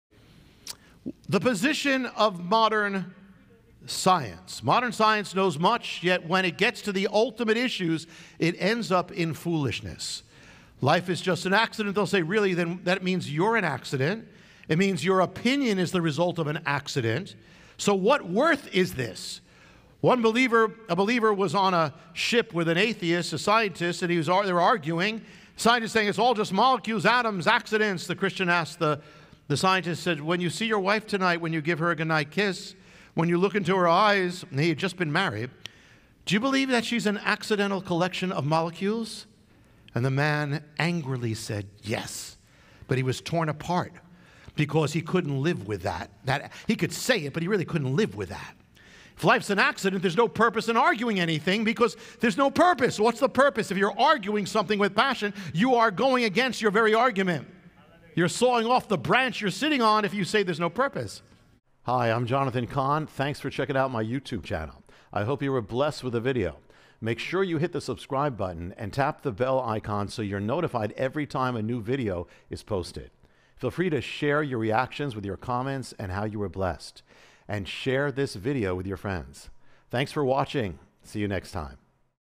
Croatian-Jonathan Cahn-The Atheist and his Wife-Ateist i njegova žena-Hrvatski
Link to original video:
Iz "Mudri ljudi i budale" (Poruka 2401)
From "Wise Men & Fools" (Message 2401)